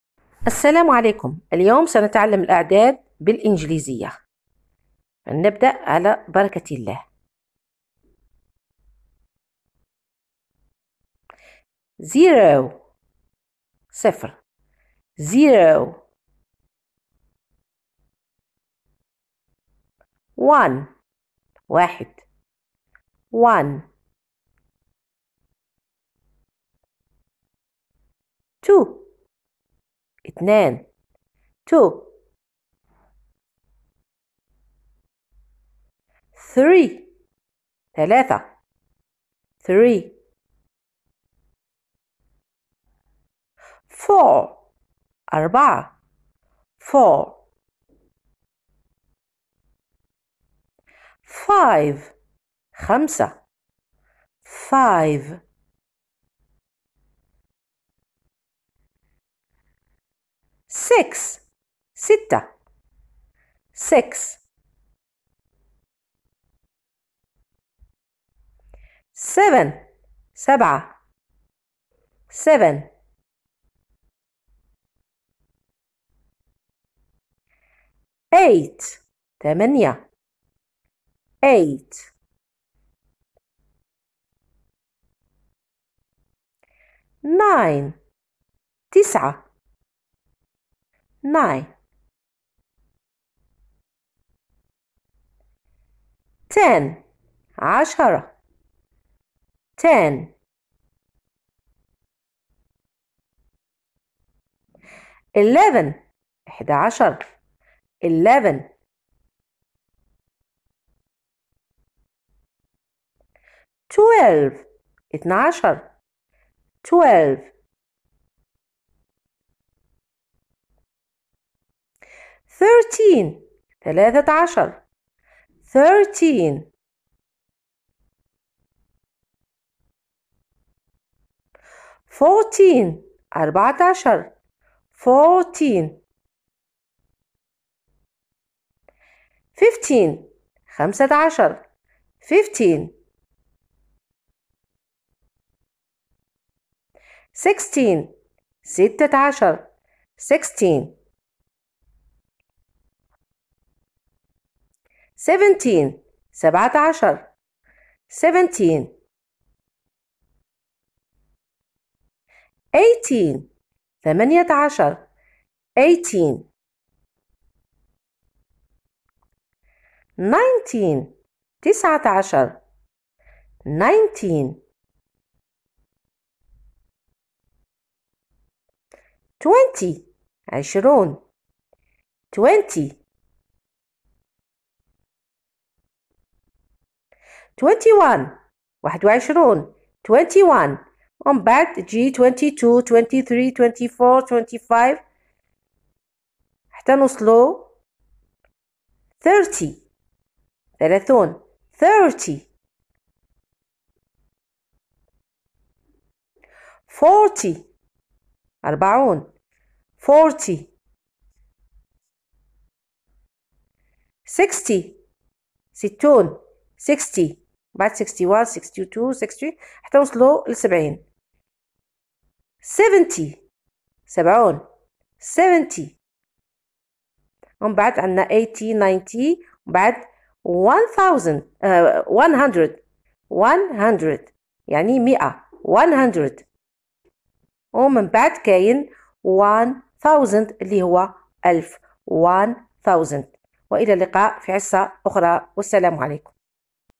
تعلم الاعداد بالانجليزية//easy way to learn English//The numbers
السلام عليكم.نتعلم اليوم الاعداد بالانجليزية بطريقة جد مبسطة .فرجة ممتعة.